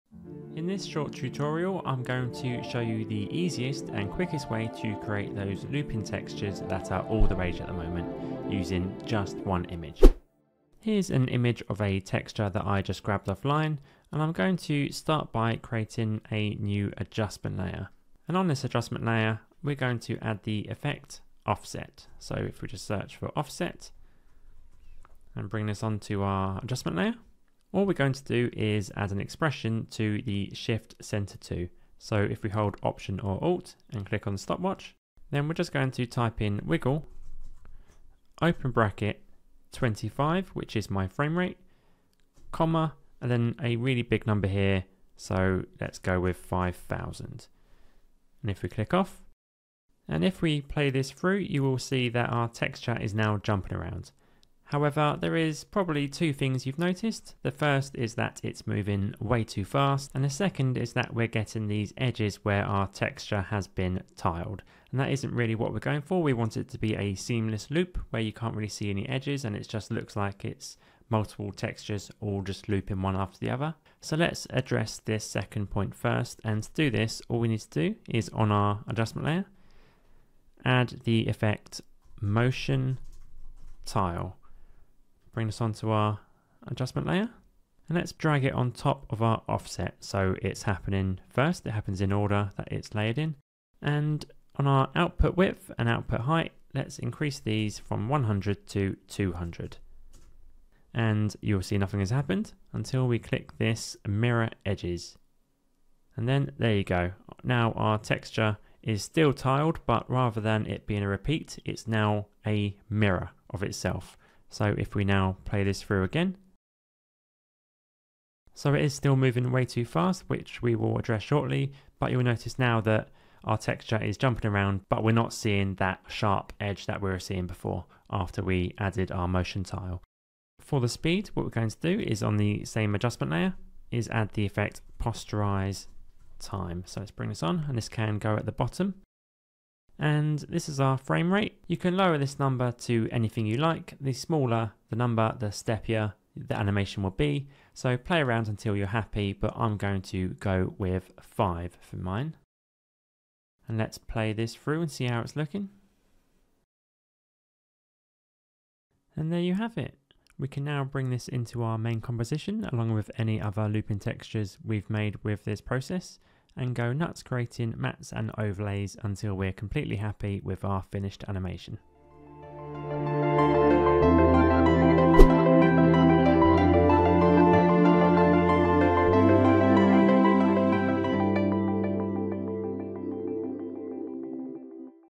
Create a Looping Texture in After Effects
In this quick and easy tutorial, learn how to create a looping texture in after effects using just one image or photo.

All you'll need is one texture in your chosen style, so gone are the days of finding multiple images and individually editing them in After Effects!